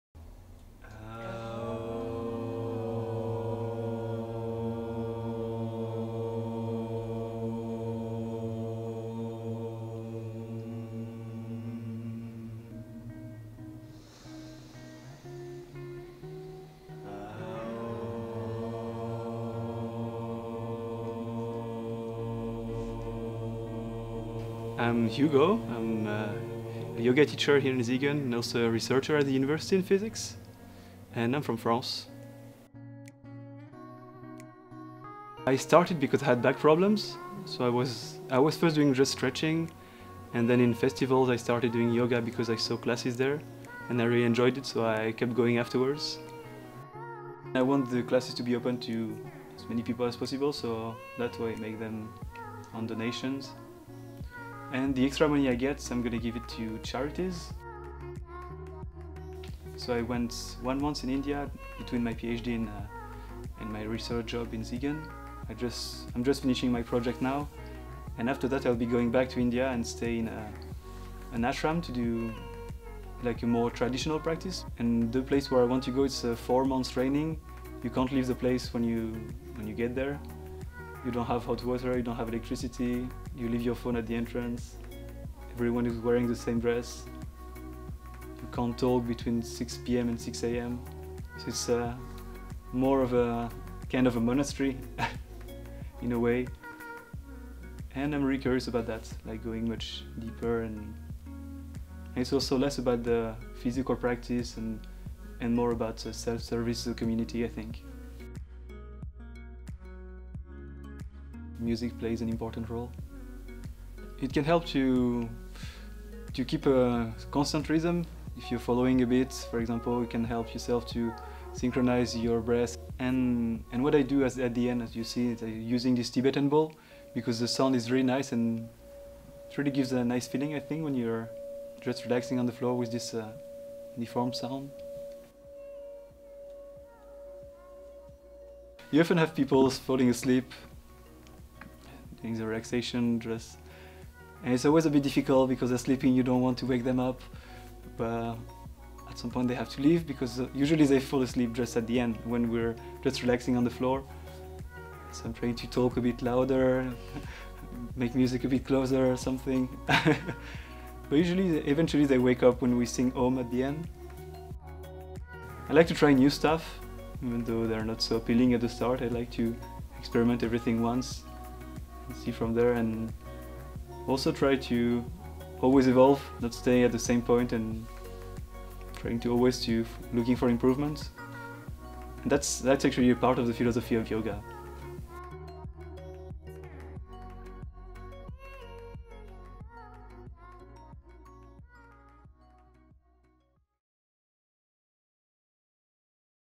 Throwback - Portrait of a Yoga Teacher (2018)
Namaste everyone!

I've been really silent lately as I ended up staying longer in India and couldn't find time to edit my videos anymore but I collected a lot of material and will be back telling you more about my journey!

Meanwhile here is a video one of my Yoga student made in Dec 2018 about the classes I was running in Siegen, Germany on the side of my academic research in Particle Physics.
Enjoy discovering this different aspect of myself!

PS: The name mentioned in the video is my old one ;)

Travel

Musique

Yoga